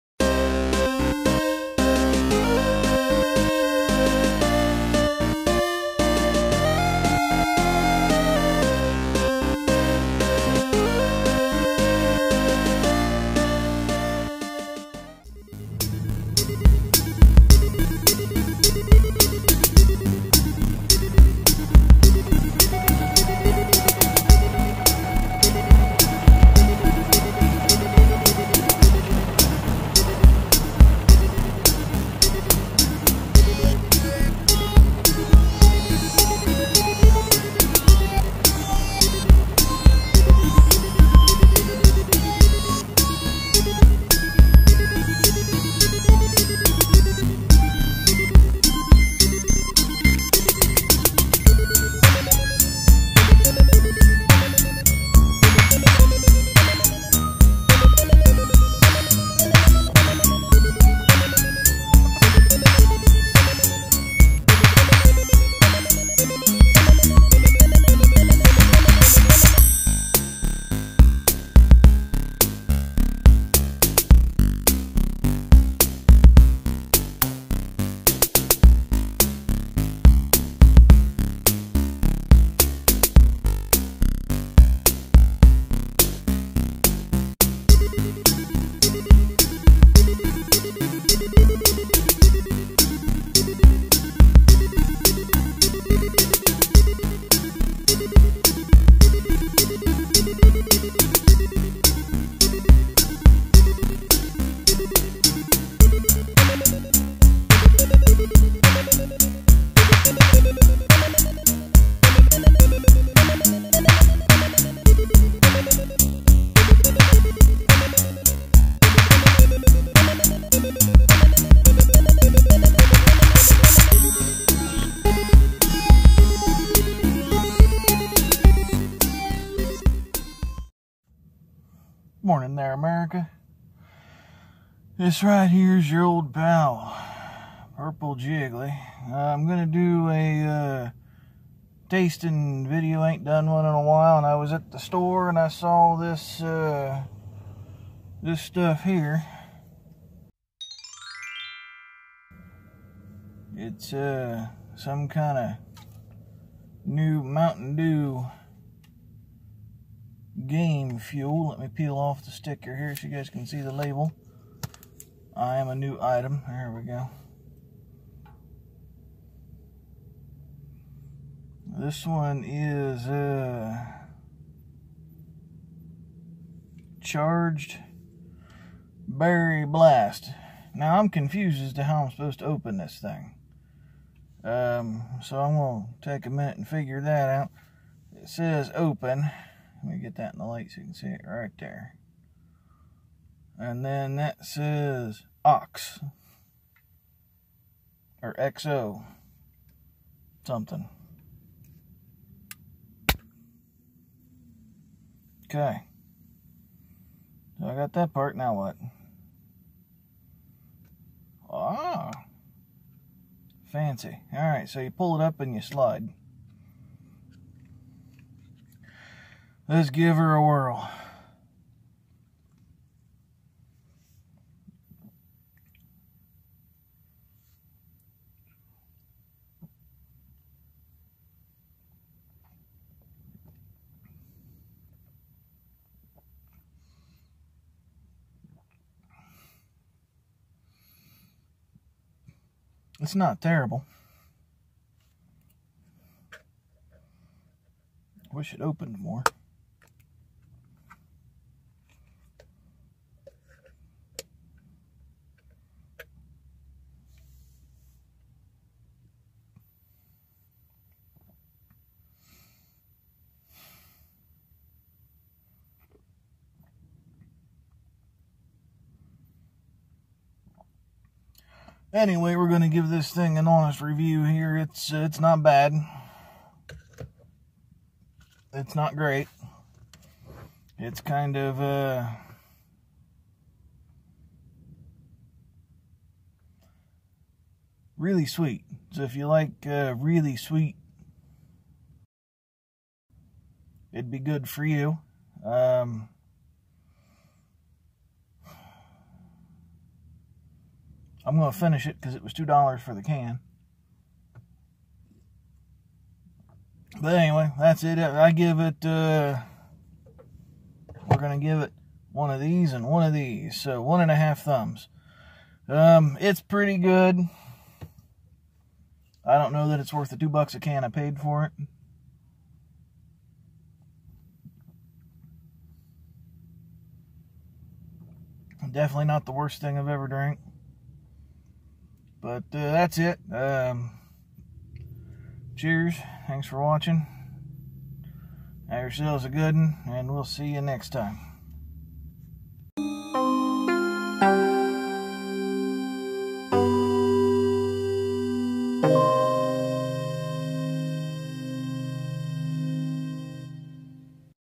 Mt. Dew +AMP Game Fuel Charged Berry Blast
Jiggly tries new Mt. DEW Game Fuel. He gives a bare bones honest review.